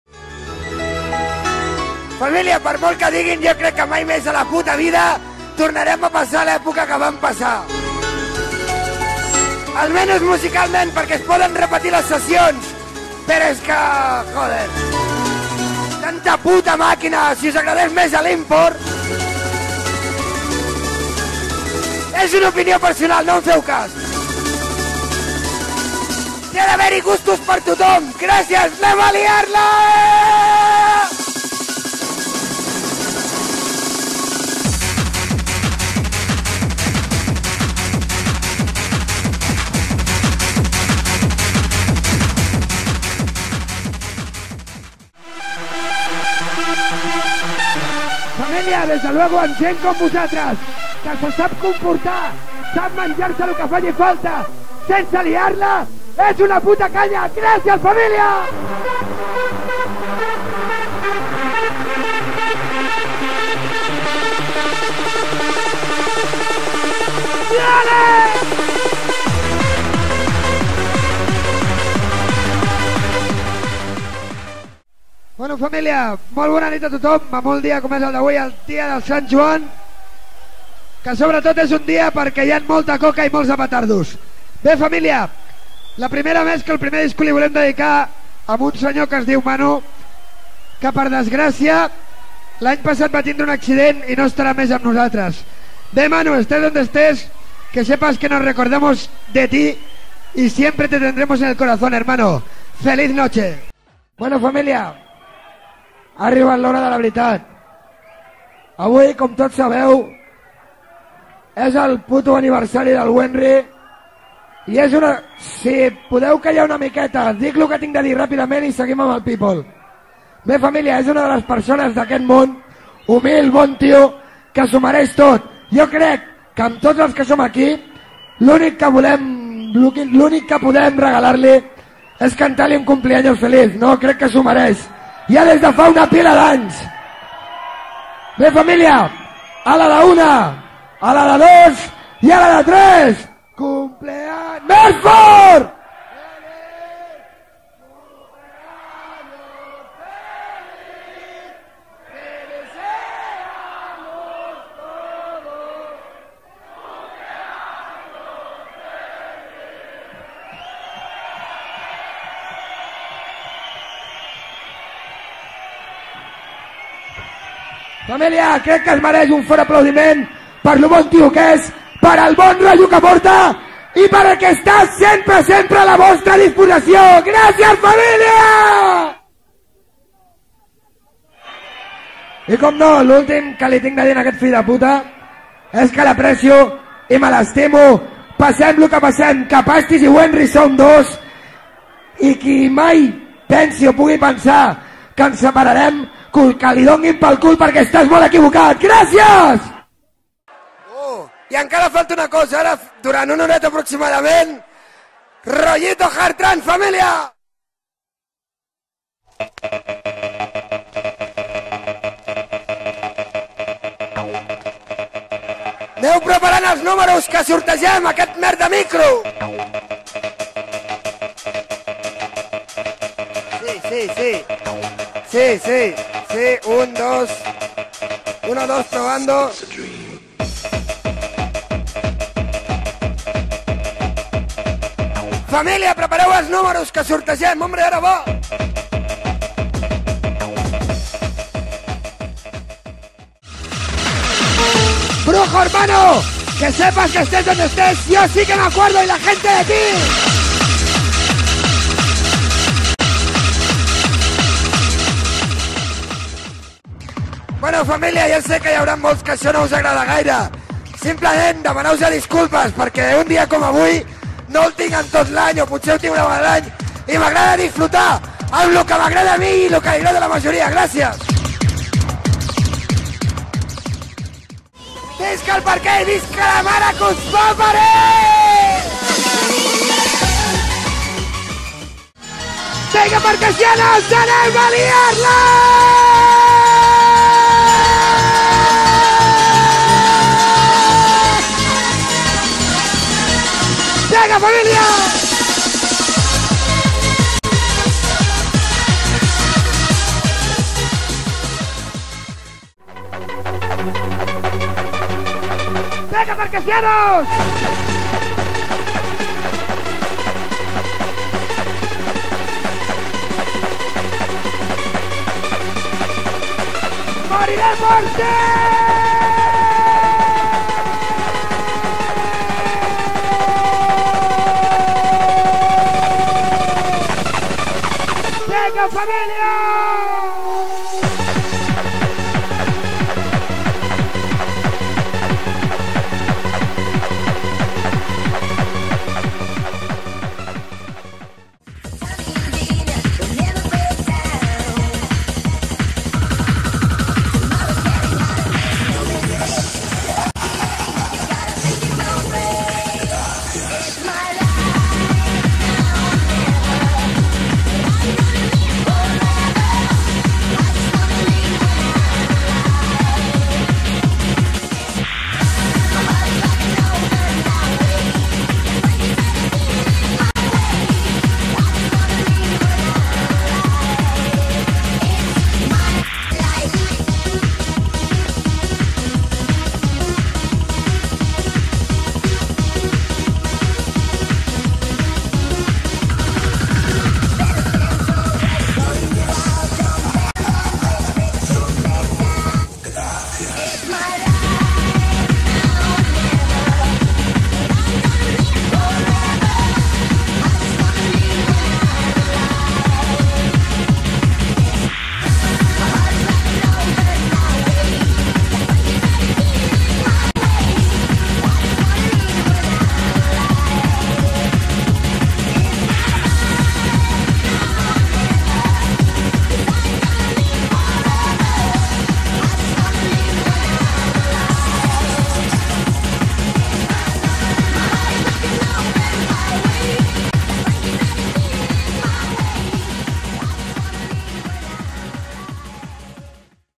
Dj PASTIS Mejores Frases en Cabina (XQUE) Vol.1 2000-2007
Recopilación de algunas de las Mejores Frases de Dj PASTIS en la Cabina del XQUE del año 2000 a 2007!